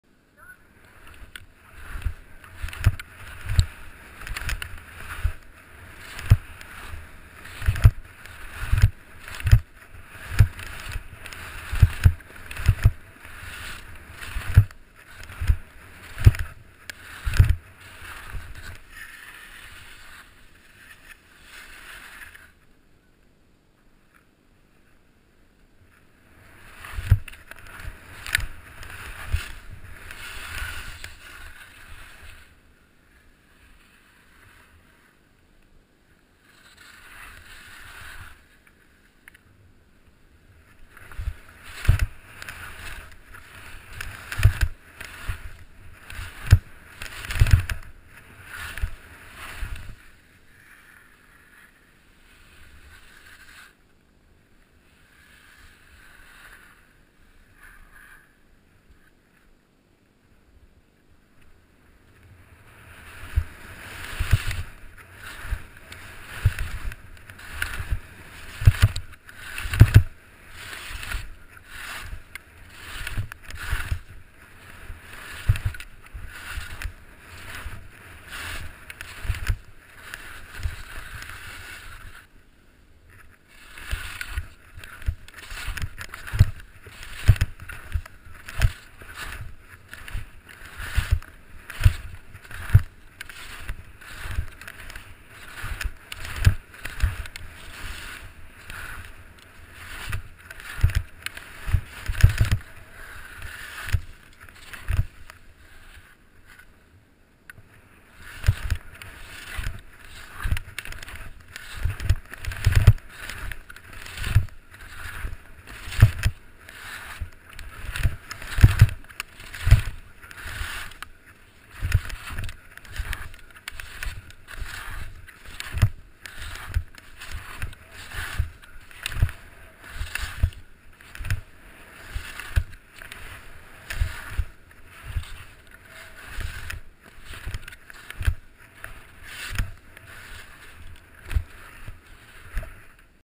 2017年3月。北海道テイネスキー場。晴れた日の北壁コブ。
天気が良く、コブの形状も見易い。
動画は、北壁コブの9割位写ってます。